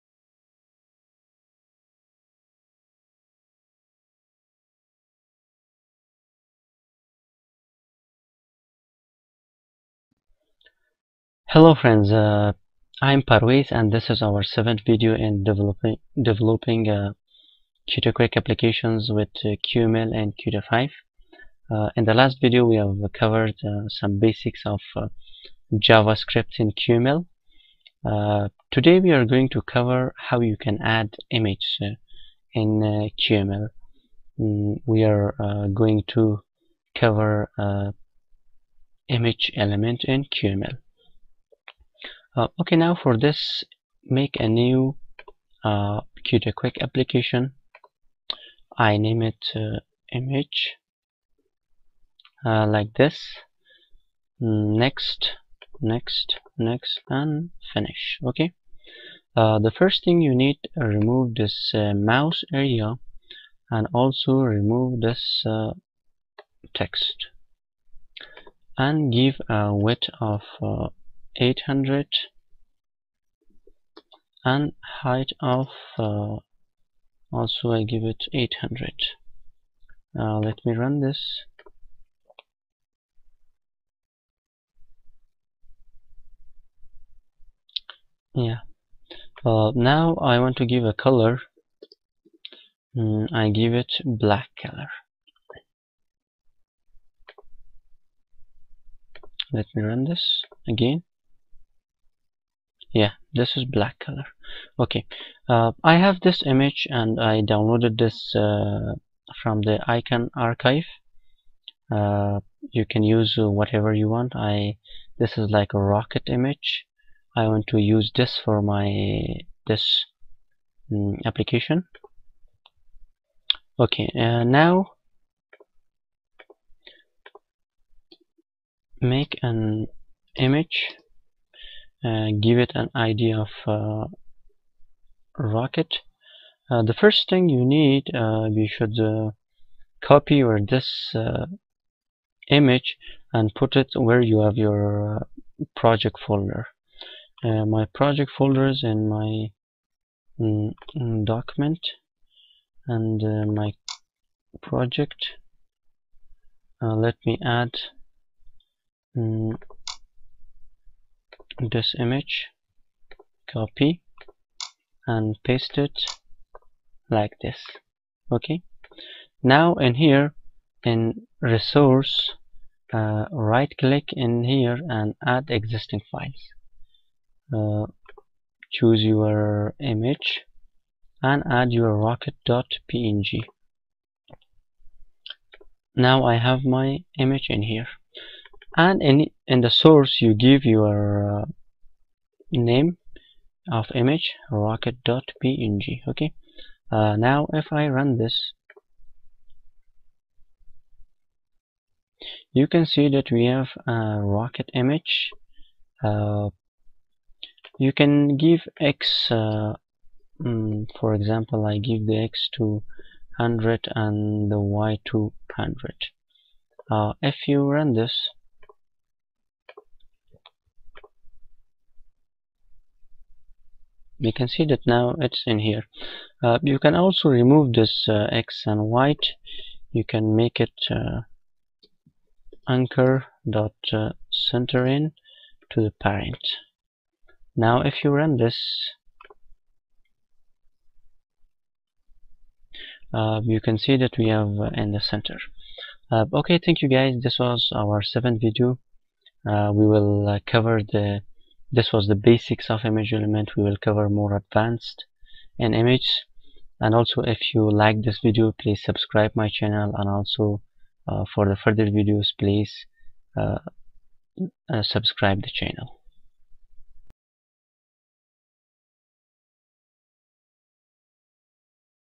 QtQuick QML Image Element #7
This Is Our 7th Video In Developing QtQuick Applications In QML
With Qt5, And In This Video We Are Going To Cover Image Element
In QML